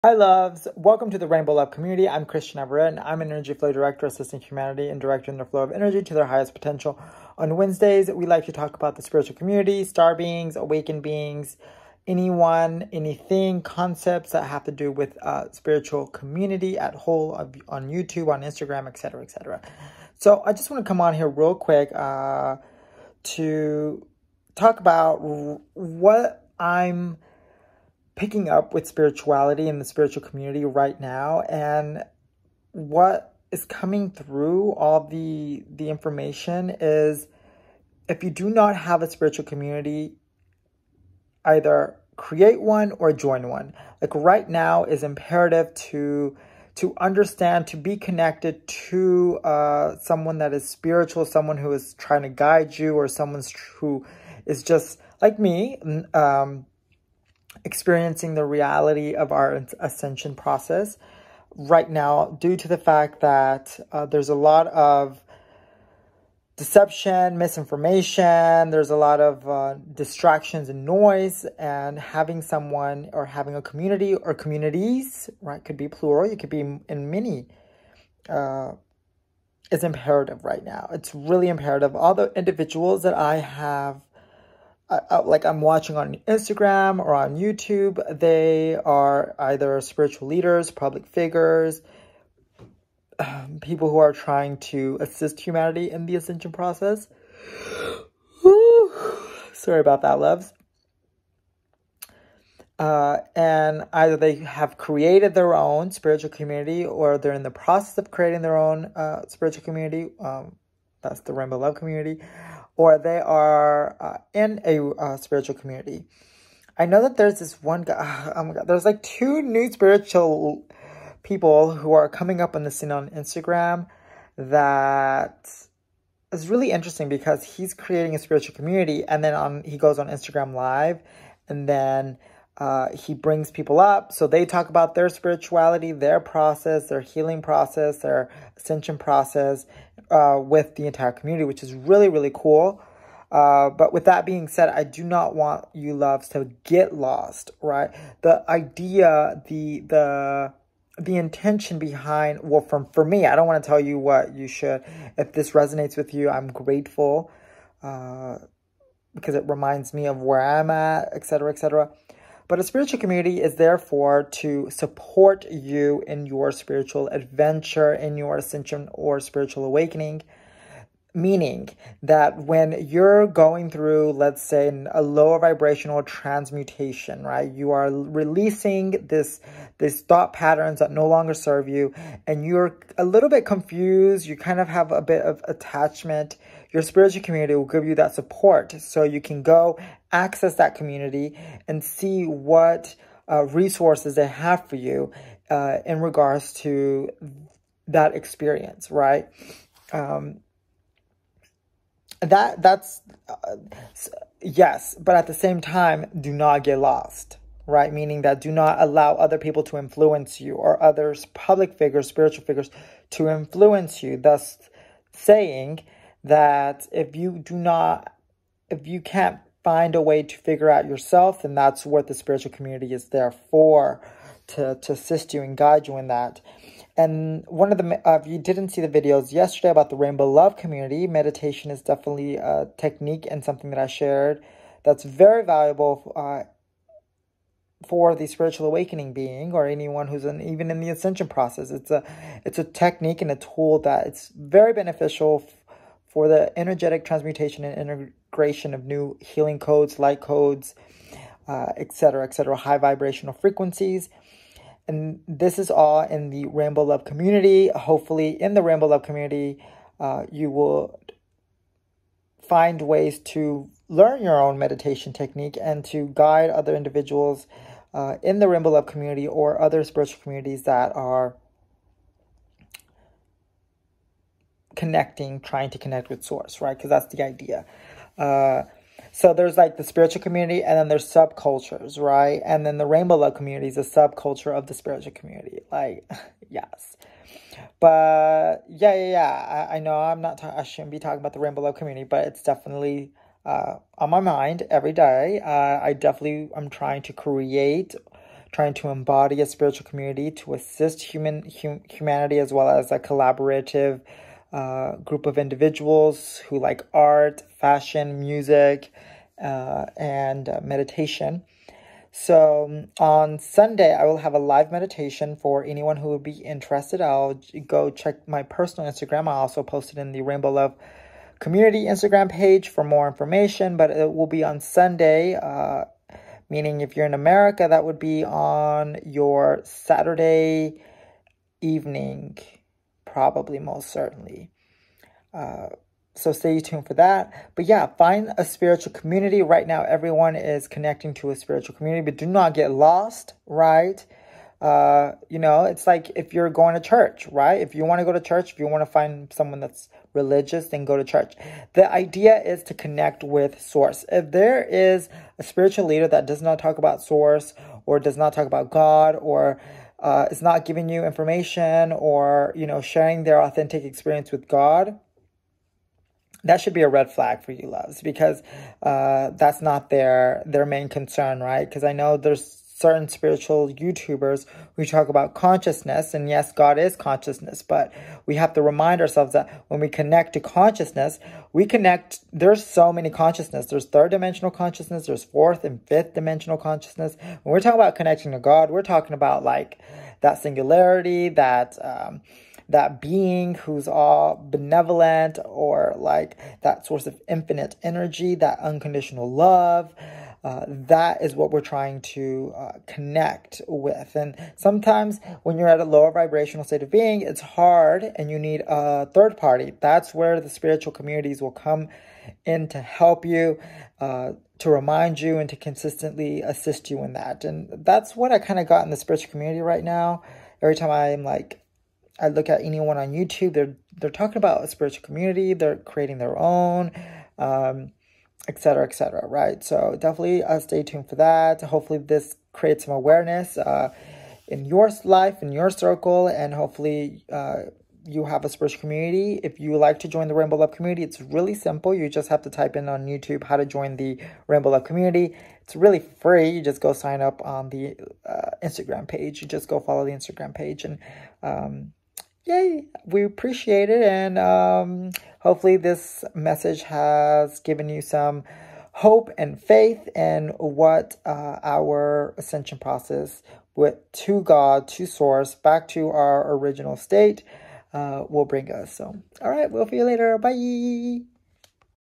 URGENT | Why you should join a Spiritual community or communities
Join a spiritual community that resonates with you.

Check out the Rainbow Love Community
Rainbow Love Community IG:

Always and forever thank you for your love and support!